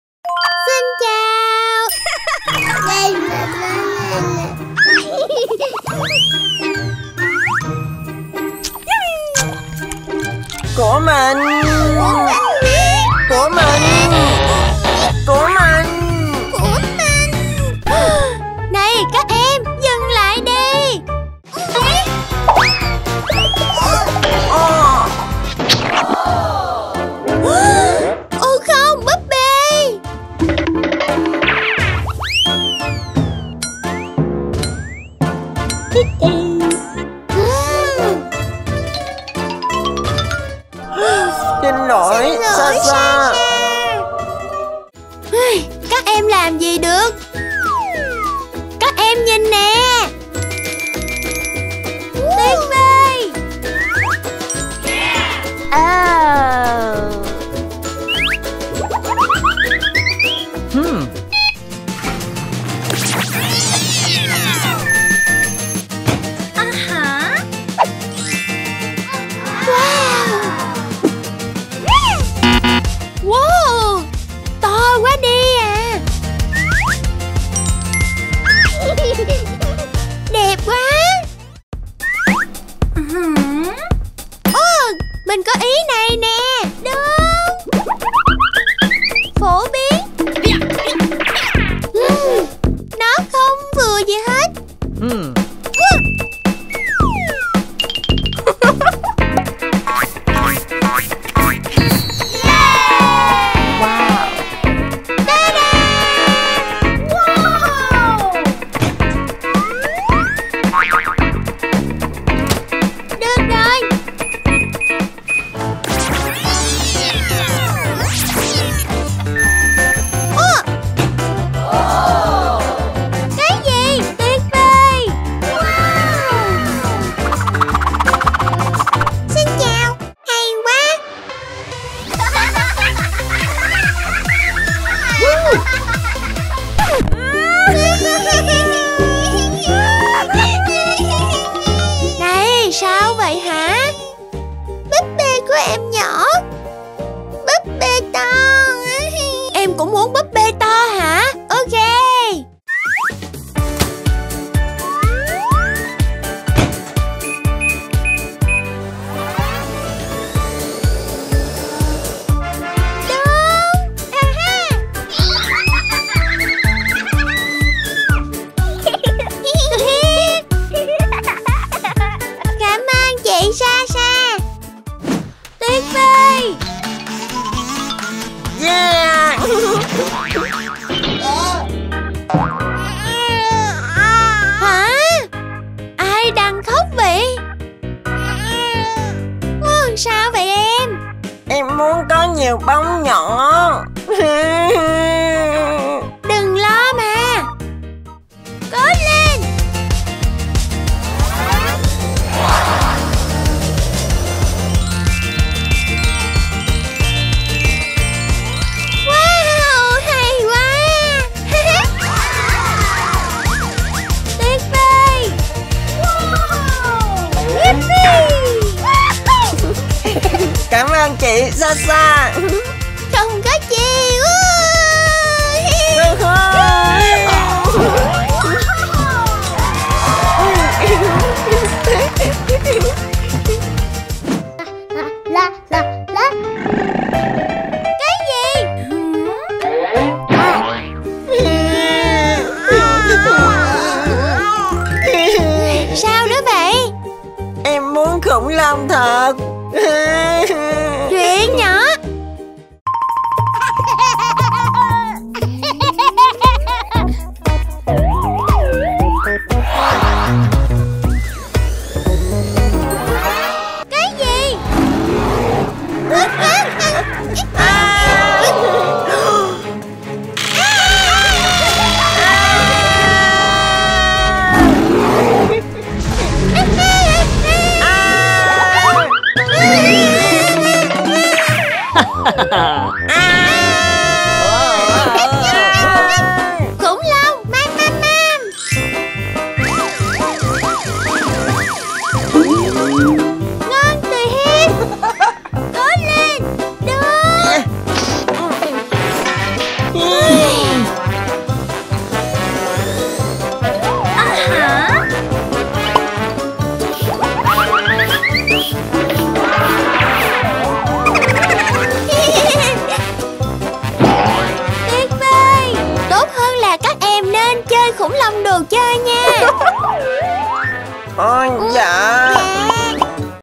Sasha và một câu chuyện vui về một chiếc máy giặt ma thuật khổng lồ dành cho trẻ em
Sasha cho Slava và Yarik thấy một chiếc máy giặt ma thuật khổng lồ biến đồ chơi nhỏ thành đồ chơi lớn.